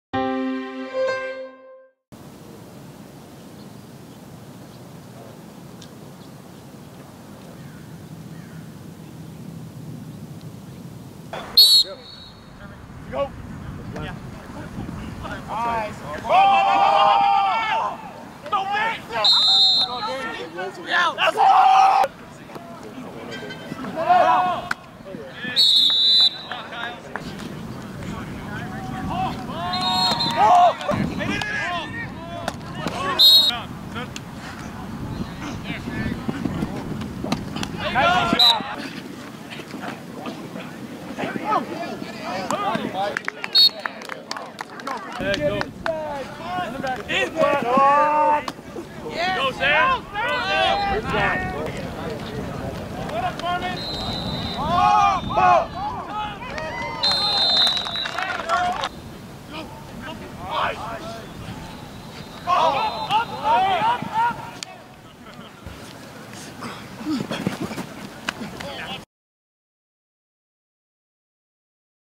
Edison passing tournament highlights
QBs Ryan Hilinski, Bryce Young, DJ Uiagalelei in action